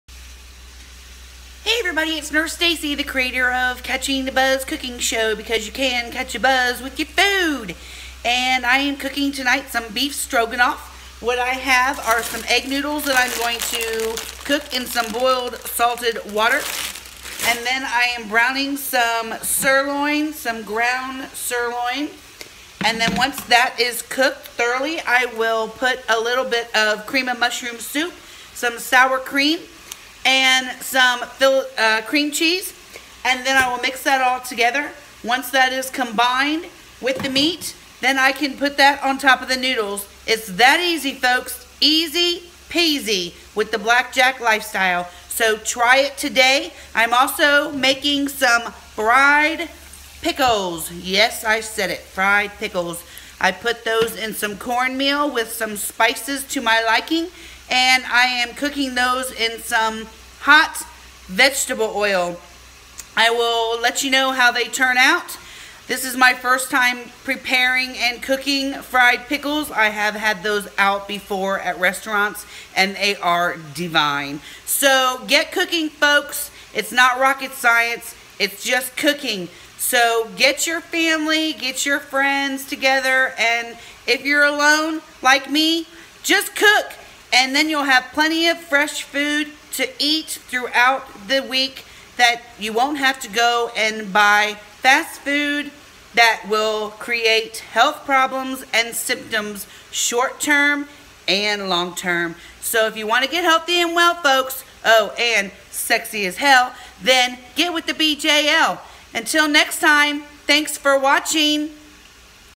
The Black Jack Lifestyle BJL
Catching a BUZZ cooking show beef stroganoff and fried pickles